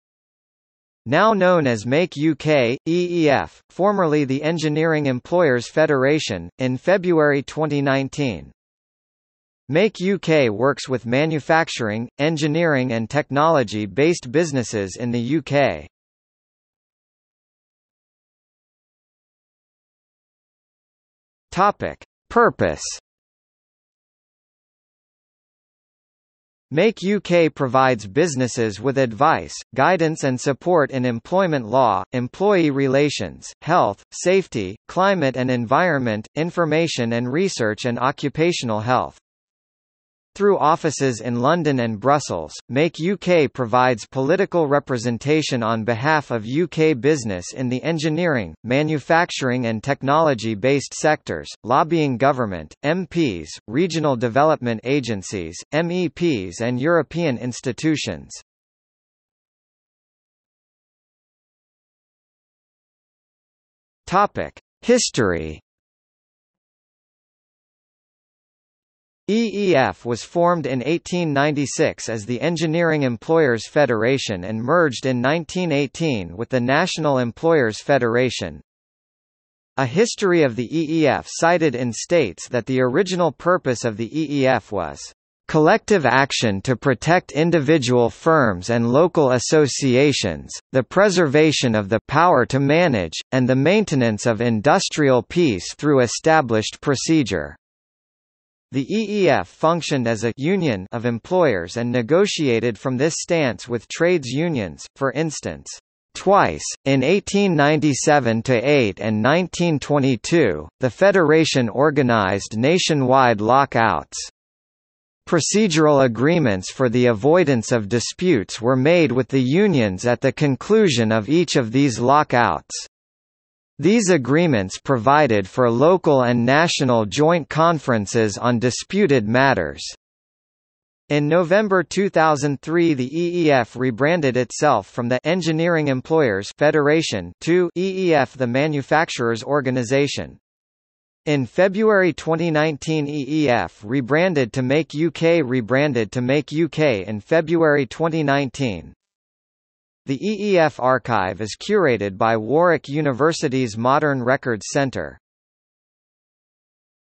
Engineering Employers Federation | Wikipedia audio article
00:00:17 1 Purpose
00:01:00 2 History

- Socrates

SUMMARY
Now known as Make UK, EEF, formerly the Engineering Employers' Federation, in February 2019.
Make UK works with manufacturing, engineering and technology-based businesses in the UK.